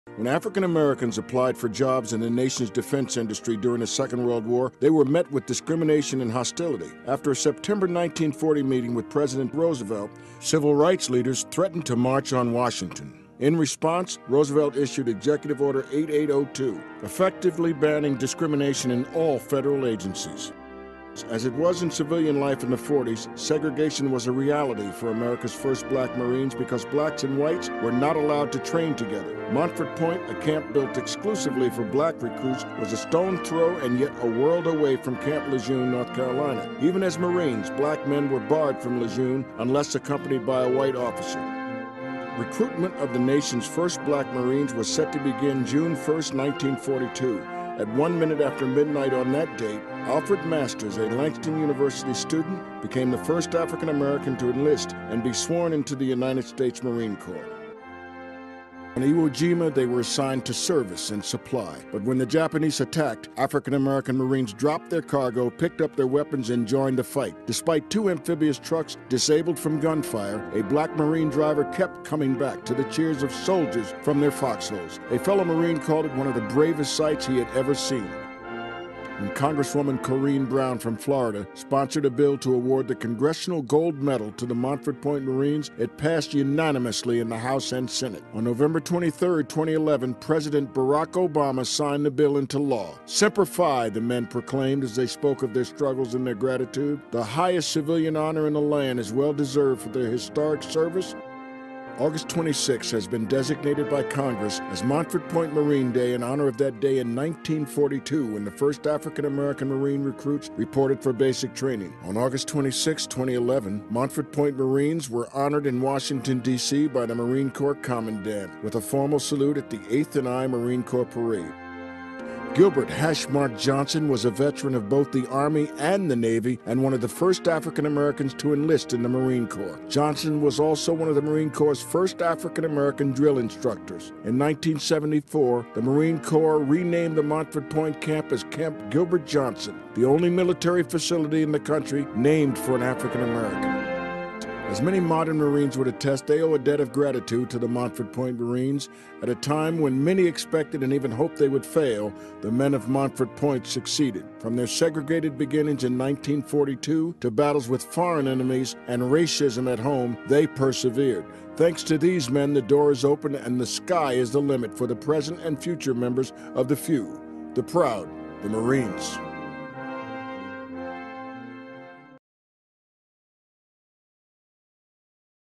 Breaking Barriers With the Montford Point Marines
The Montford Point Marines broke racial barriers in the Marine Corps during the 1940s. The impact of their efforts altered the history of the United States military. Their road through history, though paved with pain and discrimination, lead to a legacy that will be ingrained in our Corps forever.

Video by:  Cpl. Alfredo Ferrer